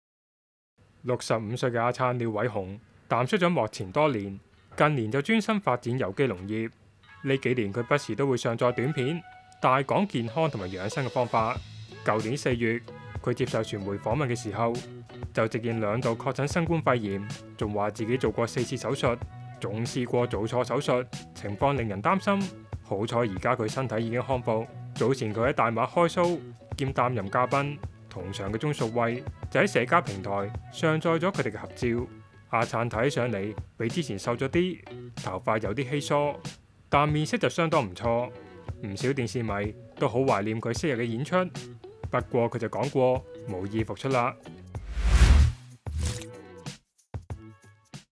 65歲廖偉雄近況曝光忽然消瘦甩頭髮 曾兩度確診新冠做錯4次手術｜01娛樂
65歲的廖偉雄「燦神」淡出幕前多年，專心發展有機農業，他種植的「亞燦米」更獲得多項殊榮。近年廖偉雄不時在個人YouTube Channel上載短片大講健康和養生方法，不過他甚至現身香港的公開活動。去年4月他接受傳媒訪問時，就直認兩度確診新冠肺炎。他又曾以「腎結石專業戶」為題發布短片，大講自己做過4次手術，更試過做錯手術，情況令人擔心，幸好現時他的身體已康復。